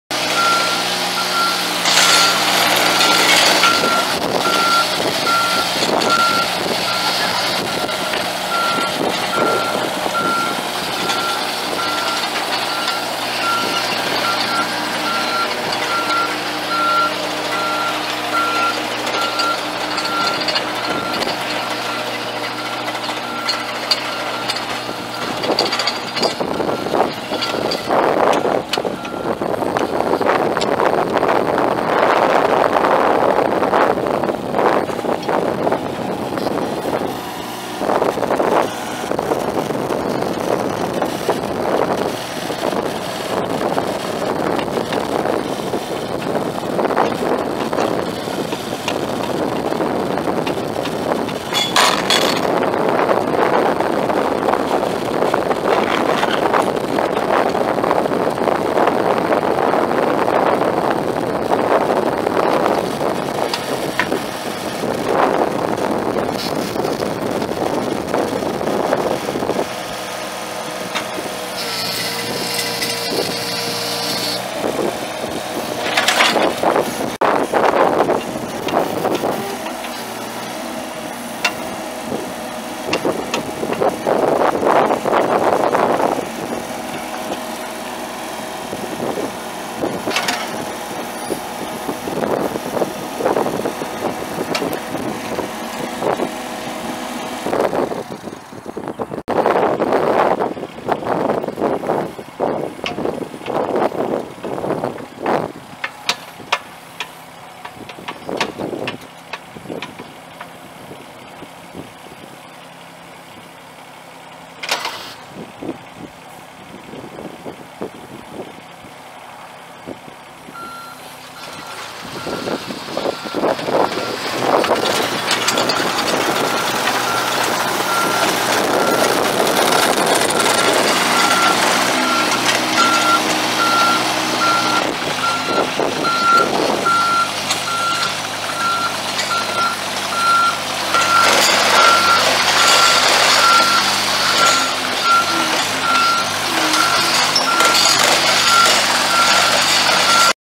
2011 John Deere 17D mini excavator for sale at auction | bidding closes October 25, 2018
2011 John Deere 17D mini excavator
1,847 hours on meter
Yanmar 3TNV70 0.85L three cylinder diesel engine
14.8 HP
Two speed hydrostatic transmission
OROPS
Suspension seat
Pattern changer
Front work lights
Standard boom
36.5" stick length
7'1" rated digging depth
12'10" rated maximum reach
12"W bucket
Auxiliary hydraulics
50"W backfill blade
Folding extensions
9"W rubber tracks
4,173 lbs tare weight
Serial 1FF017DXTBK221192